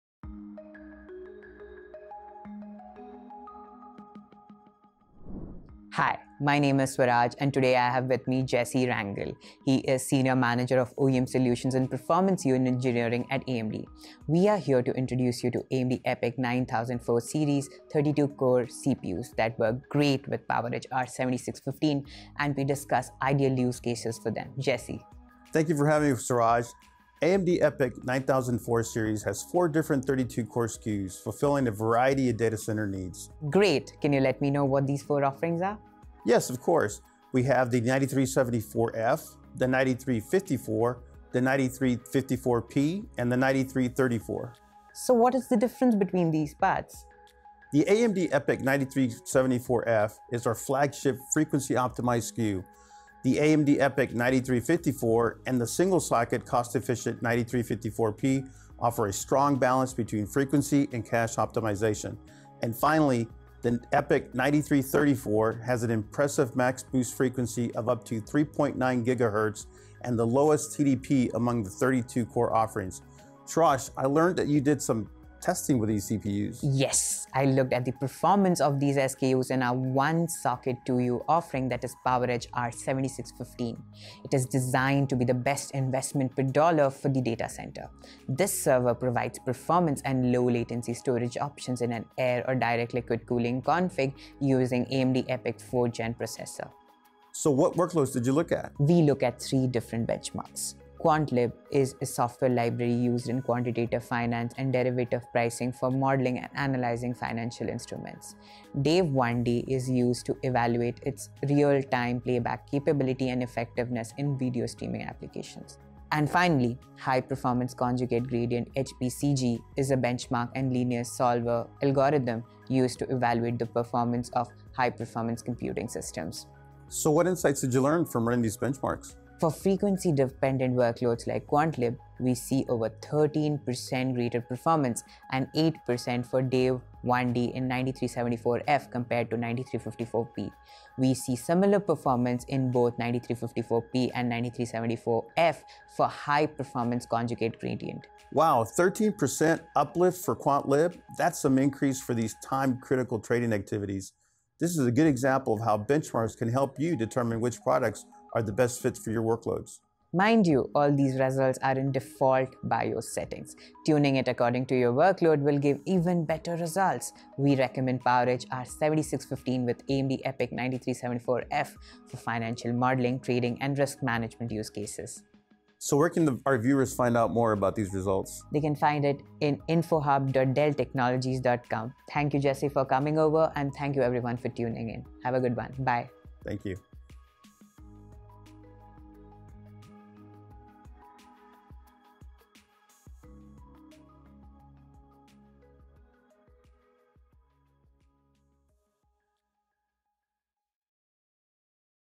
Win at FinTech with PowerEdge R7615 powered by AMD EPYC 9374F
Dell PowerEdge R7615 with 4th Generation AMD EPYC 9374F provides up to a 13% performance gain over Dell PowerEdge R7615 with 4th Generation AMD EPYC 9354P for financial trading benchmarks. This video looks at CPU benchmarks for three R7615 32-core based CPU configurations and highlights key features that enable businesses enterprises to host different workloads.